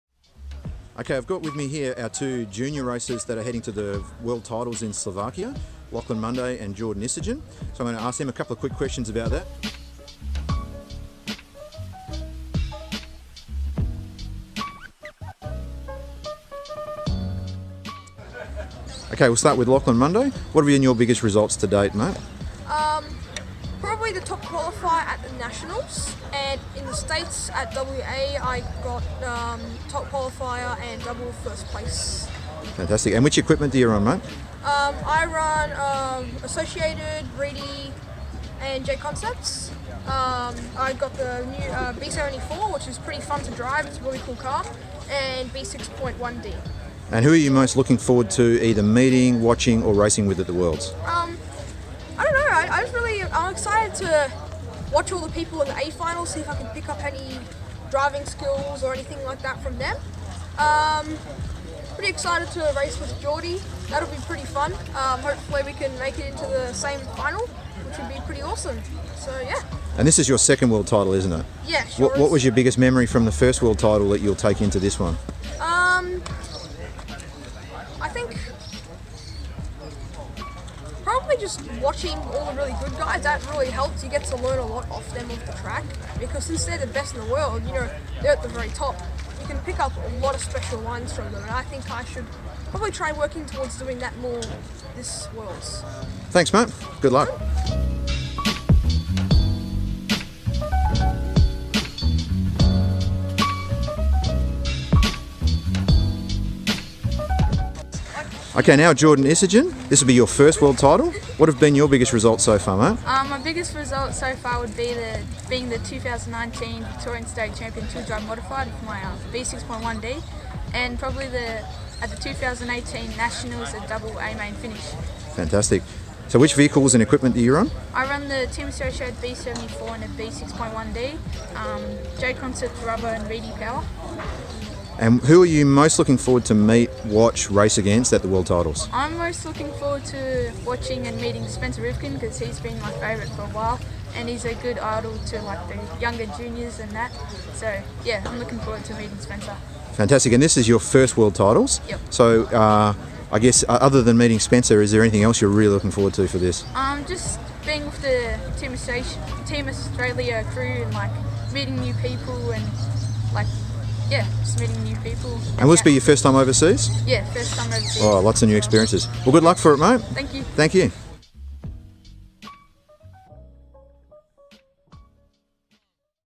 KEORCA Junior Worlds Interview
KEORCA members Ray Munday, Andrew Selvaggi, Lachlan Munday, Jordan Isergin and Jon Philpott head off to Slovakia to represent Team Australia at the 2019 IFMAR 1/10th EP OFR World Championships (14-21 September). Here we interview our two youngsters Lachlan and Jordan about their pending trip. We wish them all the best of luck at the Chmapionships.